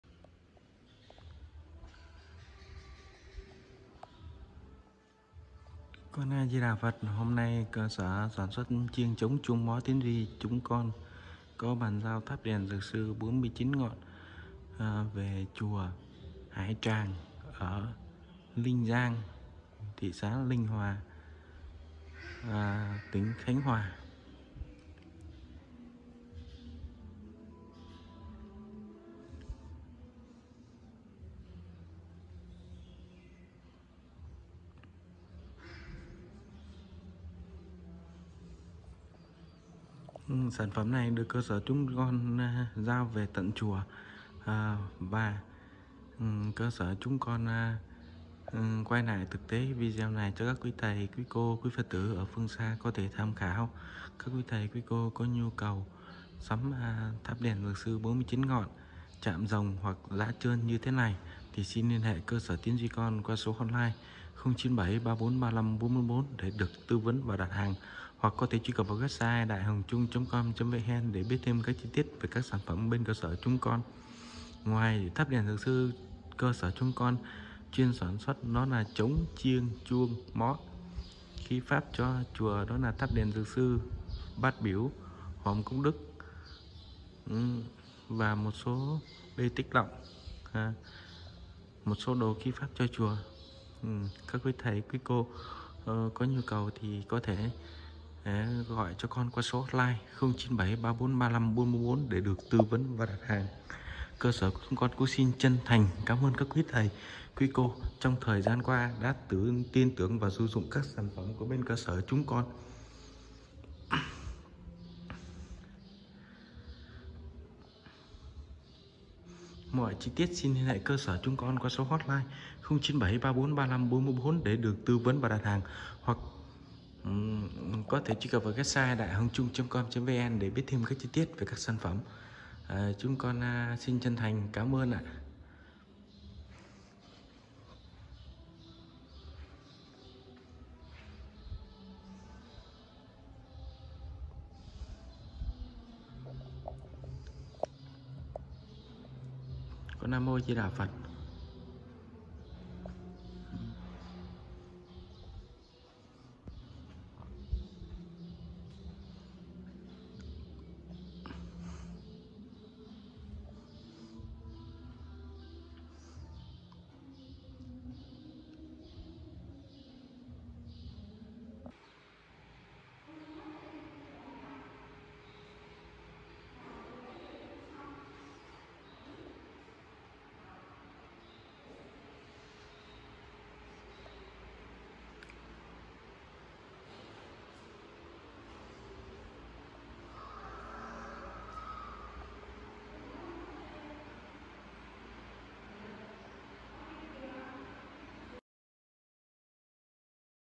Bán 49 Ngọn Đèn Cúng Phật Dược Sư Về Chùa Hải Tràng Thị Xã Ninh Hoà Tỉnh Khánh Hoà
49 NGỌN ĐÈN CÚNG PHẬT DƯỢC SƯ
Cơ Sở Tiến Duy chuyên sản xuất Tháp 49 Ngọn Đèn Cúng Phật Dược Sư bằng gỗ lim, căm xe, gõ đỏ chạm khắc rồng tinh xảo sắc nét có thể dùng để đốt đèn dầu, đèn lưu ly, đèn sáp dùng để phật tử dâng đèn cầu bình an.

 Đây là Tháp 49 Ngọn Đèn Cúng Phật Dược Sư cơ sở Tiến Duy sản xuất theo đơn đặt hàng của Sư Cô Trụ trì Ở Chùa Hải Tràng ở Ninh Giang, Thị Xã Ninh Hòa, Tỉnh Khánh Hòa và đã được cơ sở bàn giao về Chùa.

Đèn 49 Ngọn này được cơ sở Tiến Duy sản xuất bằng gỗ Lim có chiều cao tổng thể là 2,1 đường kính tròn ở dưới là 1,05 mét, có 7 tầng mỗi tầng có 7 ngọn đèn được làm gỗ trơn đầu gắn bông sen bằng gỗ, trên đỉnh có phật tử sư bằng gỗ, dưới chân được chạm khắc 3 chân hình nguyên con lân chụm lại tạo lên thế vững chắc.

Địa chỉ trụ sở chính: 621 Quốc Lộ 1K, Tân An, Phường Tân Đông Hiệp, Thành Phố Dĩ An, Bình Dương.